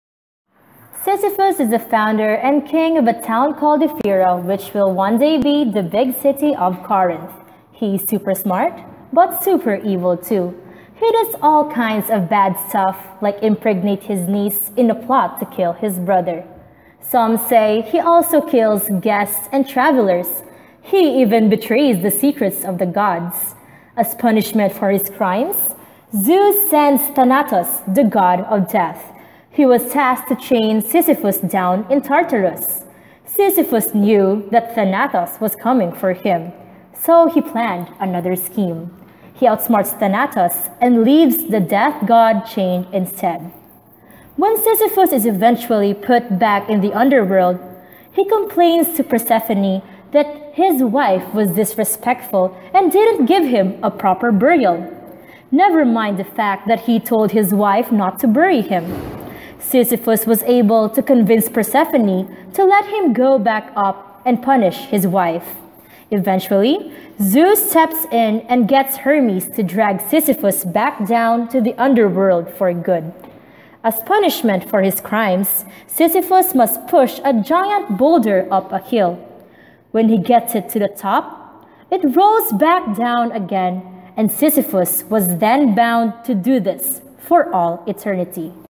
The Story of Sisyphus
Written by Nobel Laureate, Albert Camus, The Myth of Sisyphus introduces the concept of philosophical absurdity, which is man’s forever search for its meaning in life. In this story, Sisyphus, as a result of his disobedience to the gods, was forced to repeatedly roll a boulder up a hill only to watch it roll back down. This demonstrates Camus’ point in human’s search for an inherent meaning in life and their mere existence, meaningless . . .